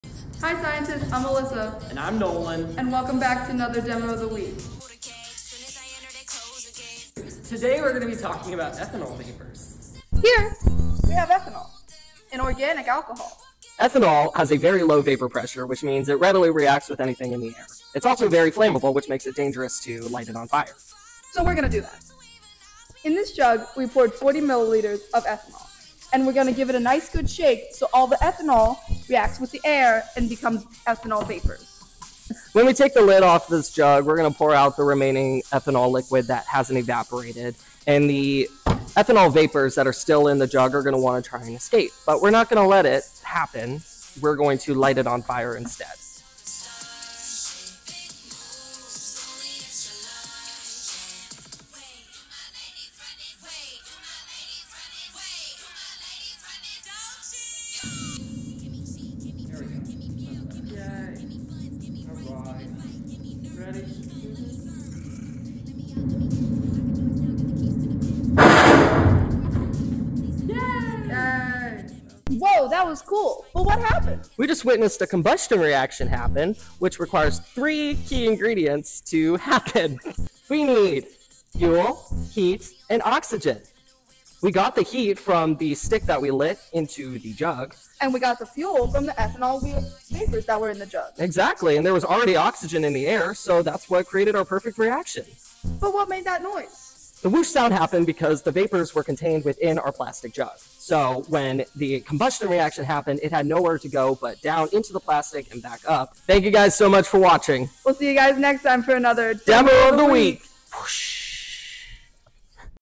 Demo of the Week: Whoosh Bottle
Hi Scientists! For today's Demo of the Week we are looking at the combustion of ethanol vapors. This demo shows how ethanol vapors in a confined space can be ignited to display to colorful jet-like flame.